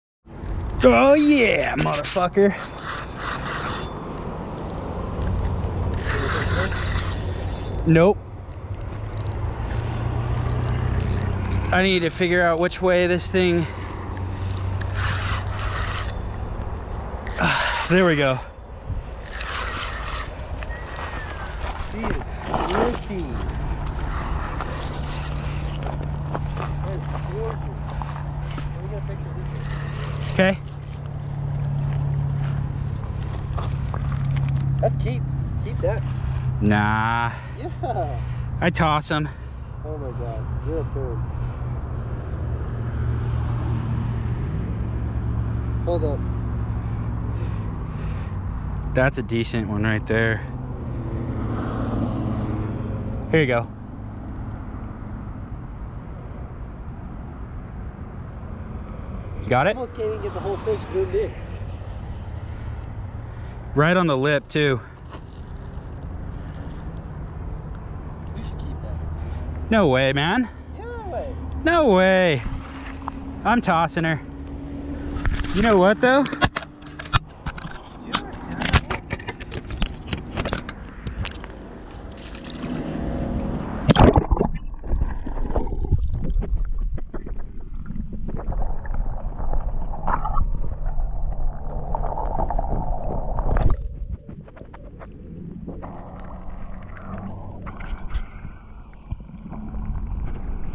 Largemouth bass eugene oregon beltline highway pond
this one was a decent size but about 20 minutes later I lost a 6 lbs bass in the weeds just 10ft down from where I was fishing. I'm going back every couple days to repeat my process to try and land a big one.

all I used was 4 split shot weights an eagle claw bait hook and a whole worm. I casted out as far as I could and let it drop down. and before I knew it I had some hogs on the end of my line.